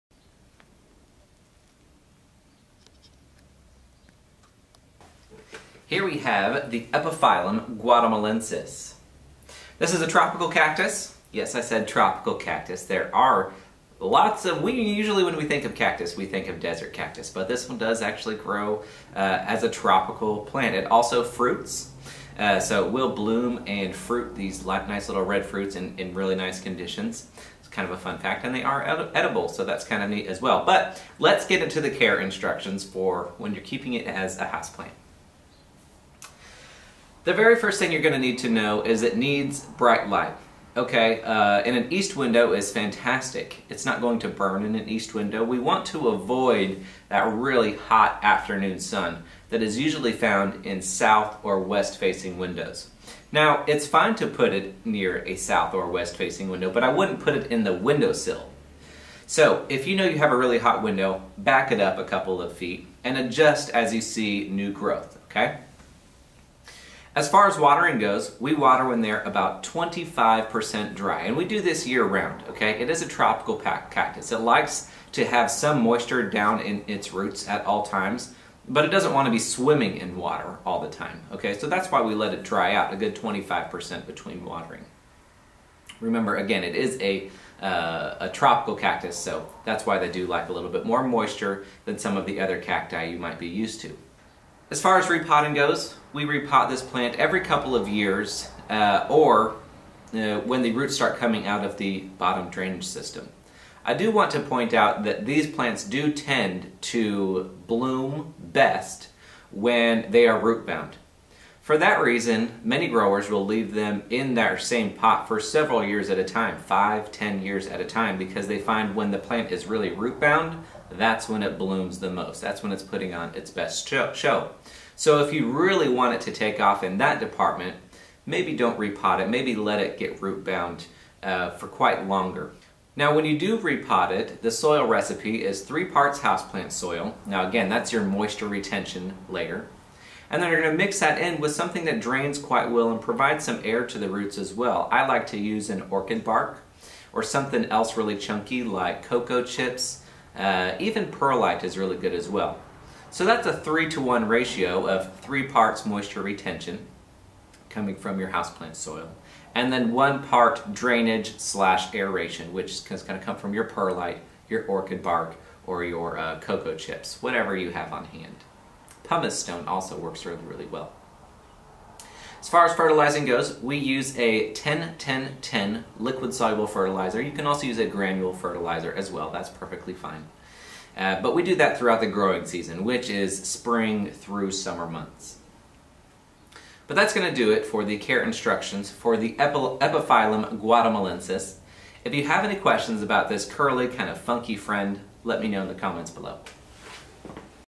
Epiphyllum Guatamalense Care (AKA) Orchid Cactus
In this video we'll go over general care for the Epiphyllum Guatamalense.